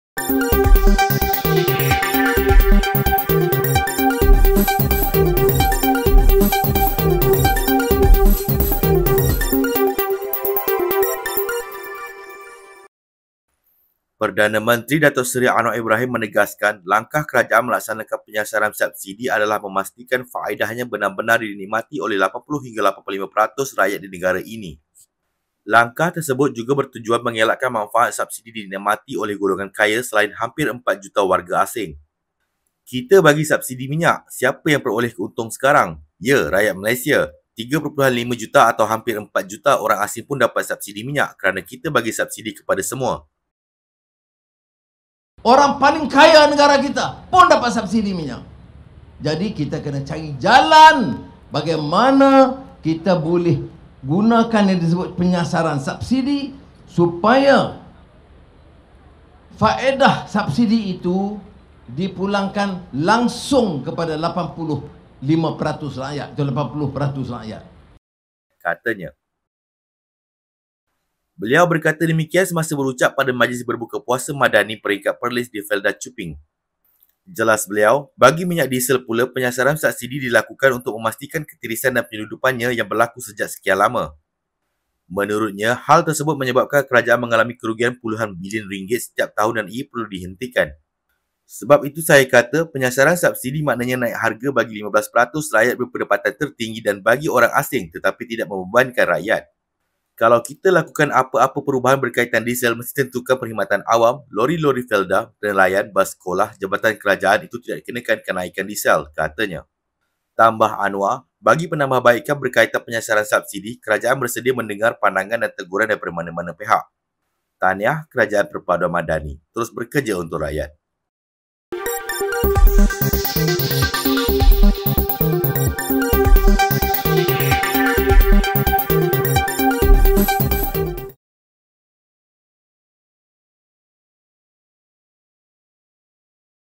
02.04.2024 - MADANI TERKINI | ELAKKAN WARGA ASING DAN GOLONGAN KAYA NIKMATI SUBSIDI
Perdana Menteri, Dato’ Seri Anwar Ibrahim menegaskan, langkah kerajaan melaksanakan penyasaran subsidi adalah memastikan faedahnya benar-benar dinikmati oleh 80 hingga 85 peratus rakyat di negara ini.